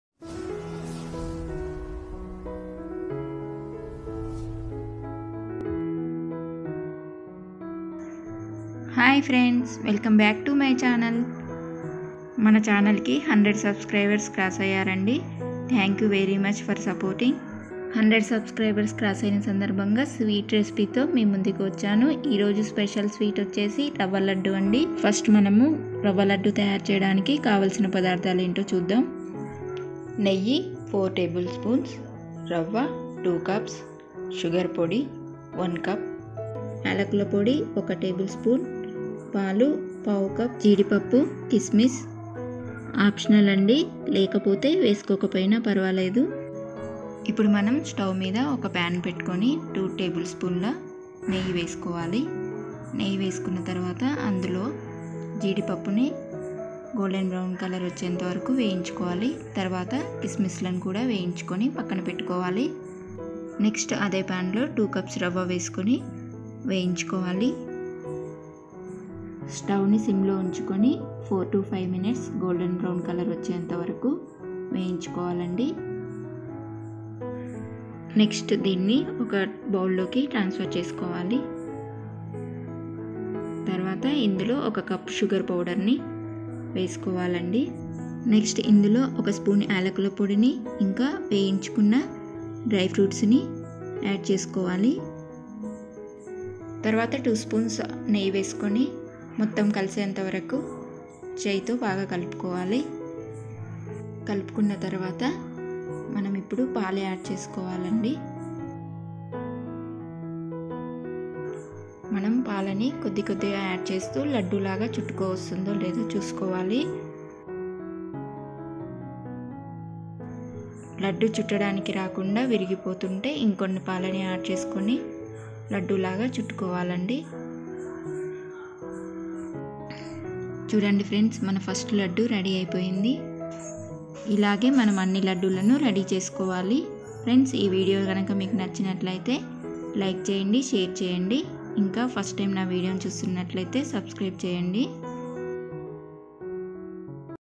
5 నిమిషాల్లోనే ఈజీ గా రవ్వలడ్డు/Suji Laddu/5 Mins Ravva laddu
5 నిమిషాల్లోనే ఈజీ గా రవ్వలడ్డు/Suji Laddu/5 Mins Ravva laddu.
Thank you very much for watching.